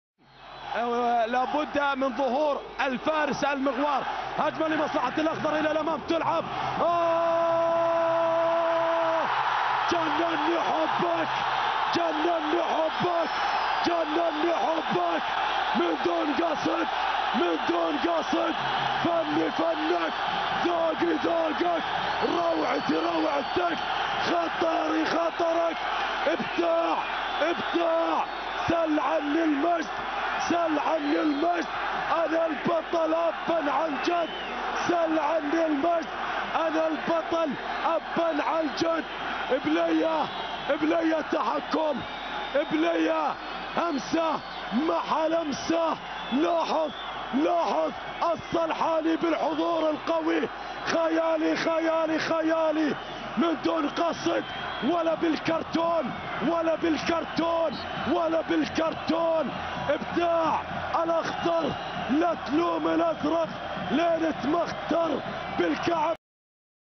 Fantastic goal in 2014 (Moataz Salhani)
Syrian player named Moataz Salhani scored fictional and historical not only happen in Cartoons